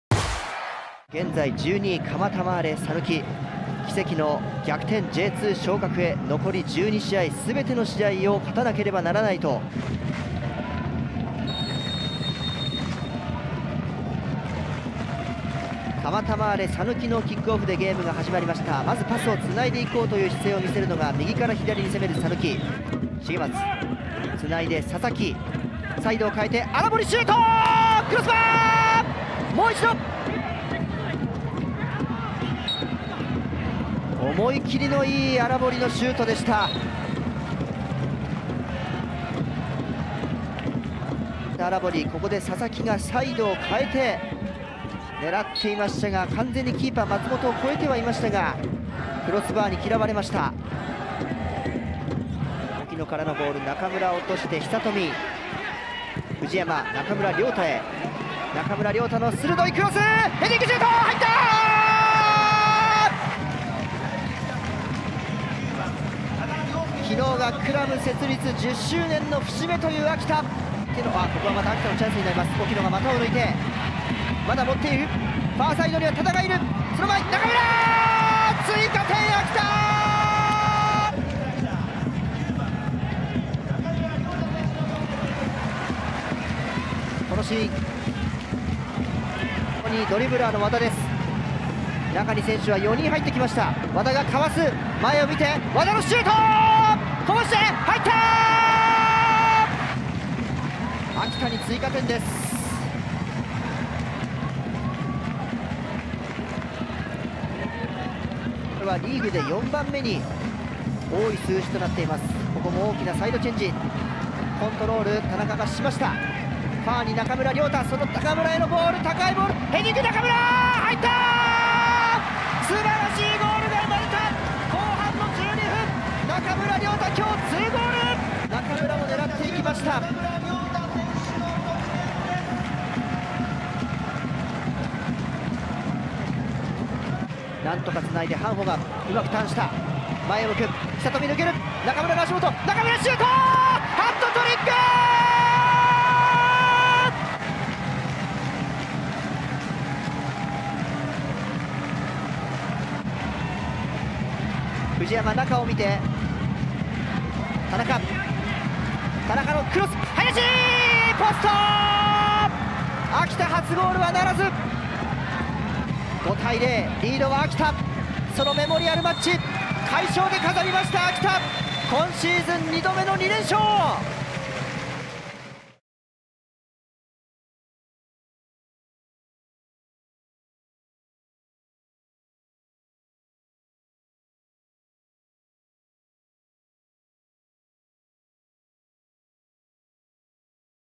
【公式】ハイライト：ブラウブリッツ秋田vsカマタマーレ讃岐 明治安田生命Ｊ３リーグ 第23節 2019/9/15
2019年9月15日（日）に行われた明治安田生命Ｊ３リーグ 第23節 秋田vs讃岐のハイライト動画です。